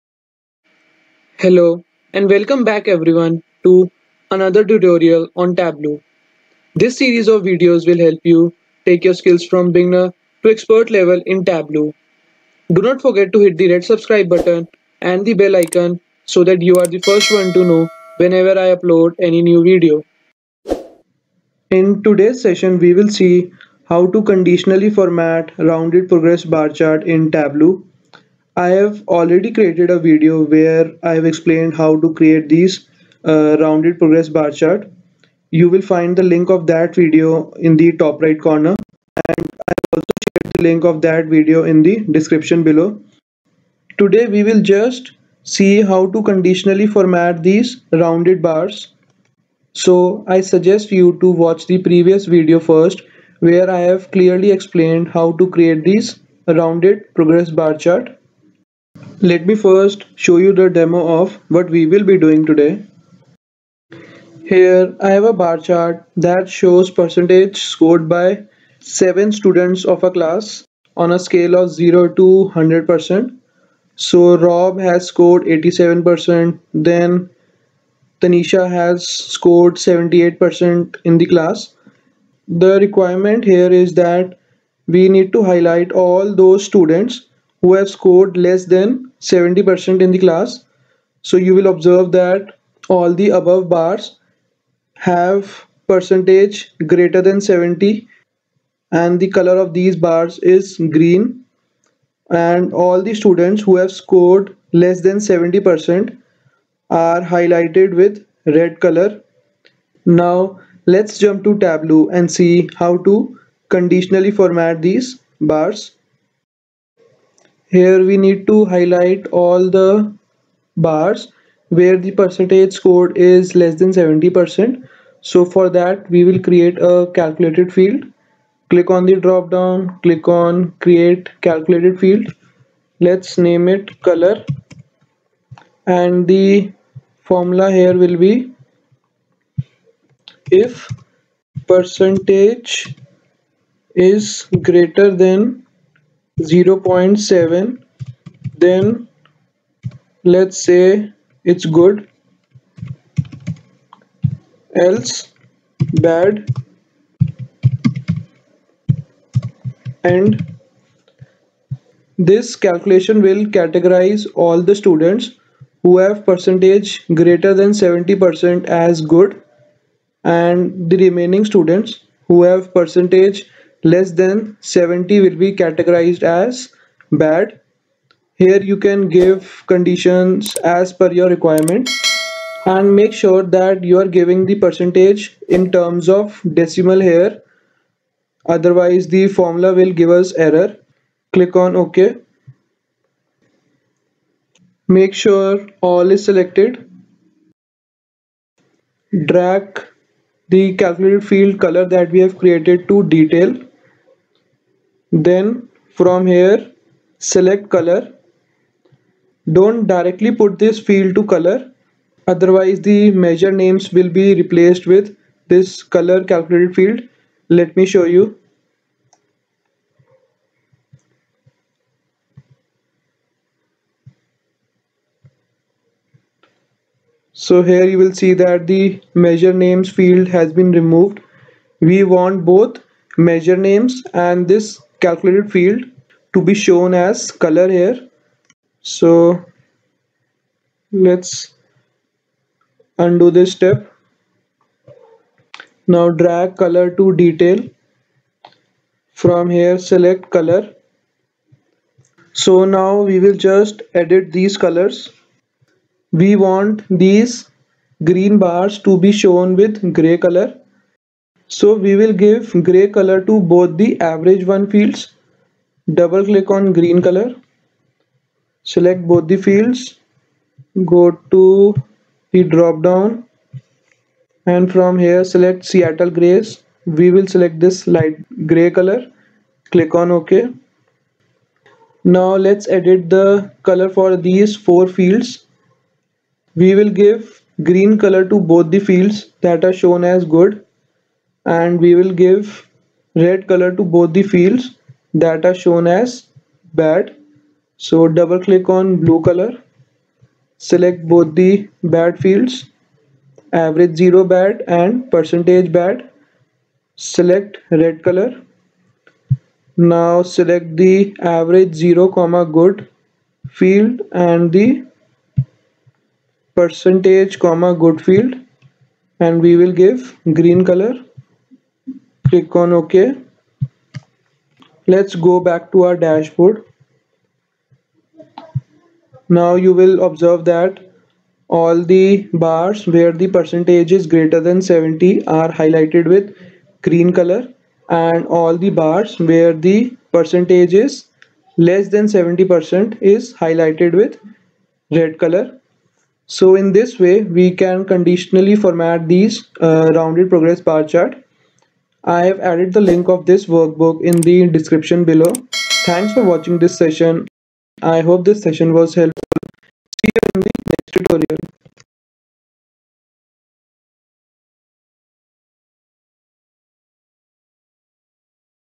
How To Conditionally Format Rounded Progress Bar Chart in Tableau
This tutorial will guide how to conditionally format rounded progress bar chart in Tableau. Progress bar chart is a good way to show what progress you have made on a scale of 0 to 100% towards a goal.